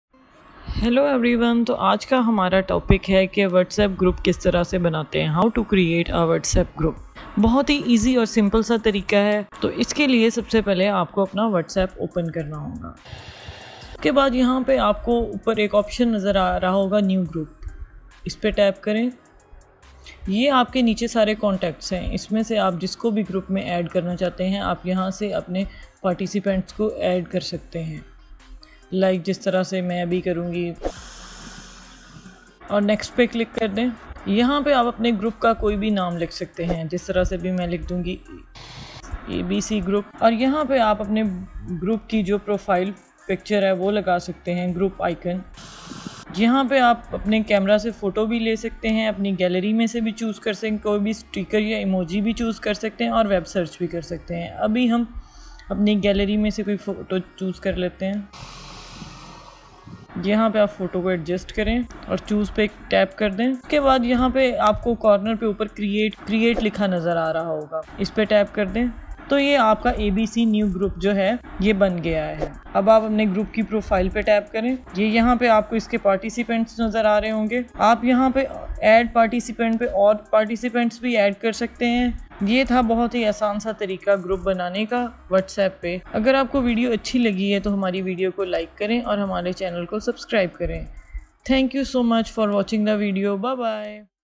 WhatsApp group kaise banaye // How to Create WhatsApp Group
Learn on, how to create WhatsApp group. In this video, you can follow step by step instructions in Urdu/Hindi to make a WhatsApp group.

00:00 - Intro
00:07 - How to Create WhatsApp Group?

Further you can follow the video tutorial to make WhatsApp group.

2 searches covered in this video:
1. WhatsApp group banane ka tarika
2. How to Setup Group in WhatsApp

ویڈیو دیکھنے کے لیے آپ کا بہت شکریہ❗